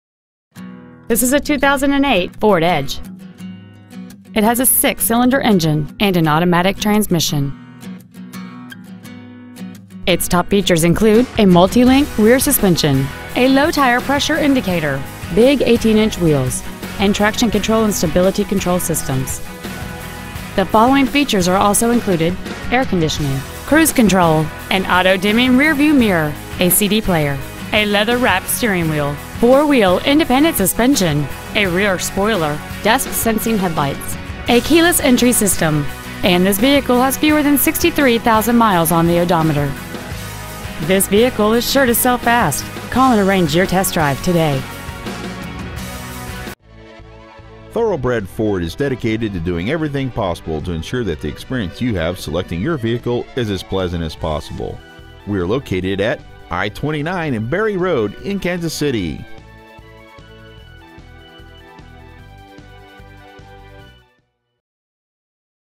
2008 FORD EDGE Kansas City MO
Year : 2008
 Make : FORD
 Model : EDGE
 Engine : 3.5L V6 24V MPFI DOHC
 Trans . : 6-SPEED AUTOMATIC
 Exterior : CREME BRULEE
 Miles : 62,602
 Interior : CAMEL
 Stock : 32099A